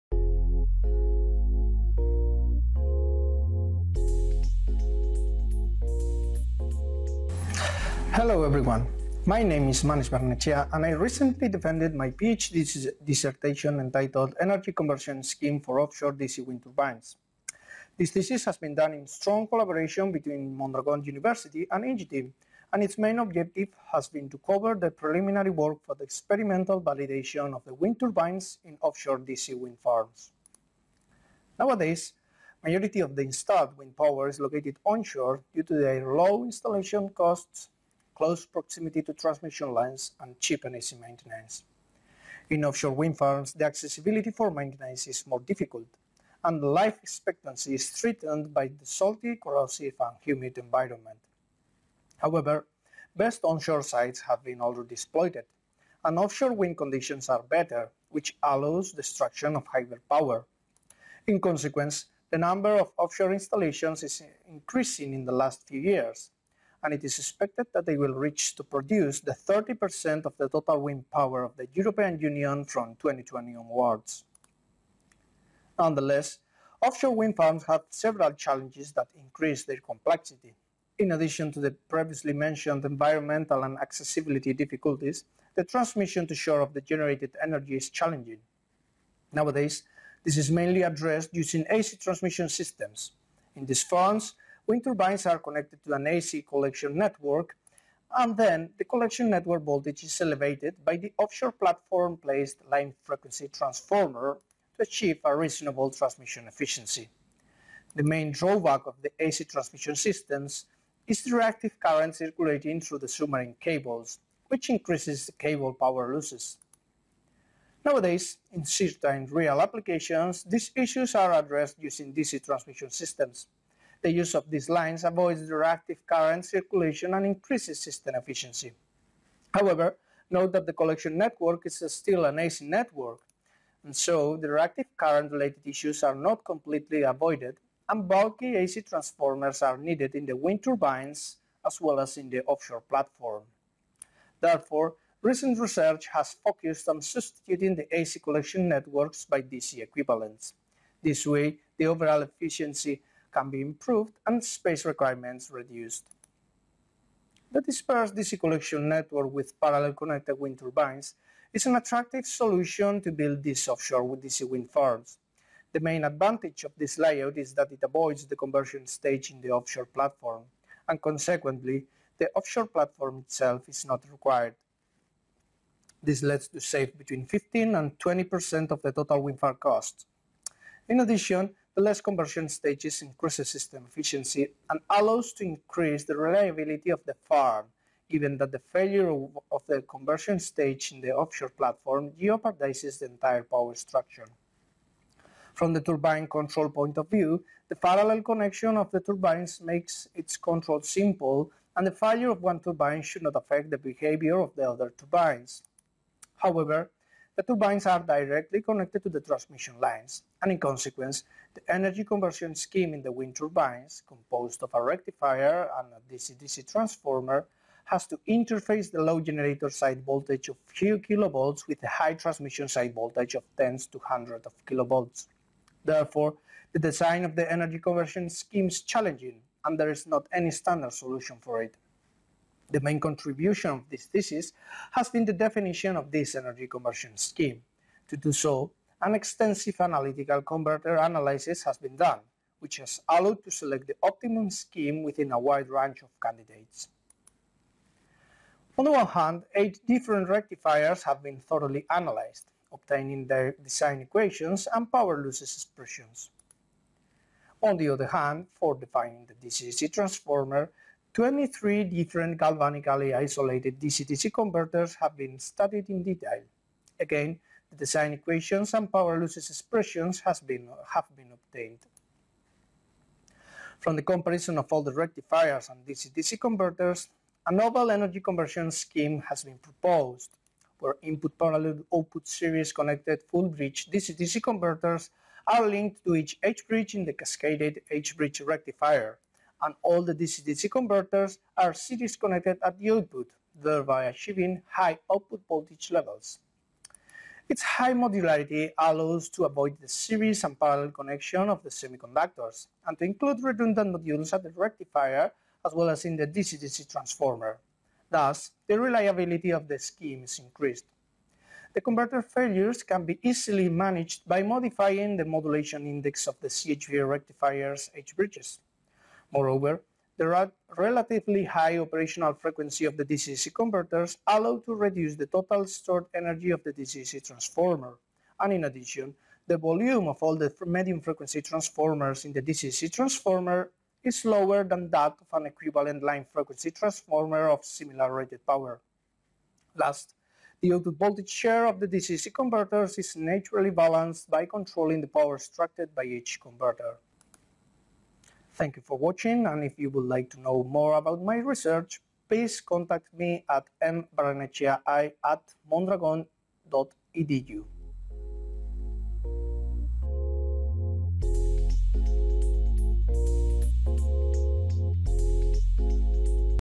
PhD Manex Barrenetxea – Energy Conversion Scheme for Offshore DC Wind Turbines
Ph.D. thesis defended on 16th May, 2016.  The main objective of the dissertation has been to cover the preliminary work for the experimental validation of the wind turbines in offshore DC wind farms.

Tesis doctoral defendida el 16 de mayo de 2016. El objetivo principal de la tesis ha sido abordar el trabajo de investigación previo a la validación experimental de las aeroturbinas de los parques eólicos marinos con redes DC.

2016ko maiatzaren 16an defendatutako tesi dokorala. Ikerketa lan honen helburu nagusia  DC saredun itsas parke eolikoetako aerosorgailuak esperimentalki balidatu aurretik egin beharreko ikerketa lana burutzea da.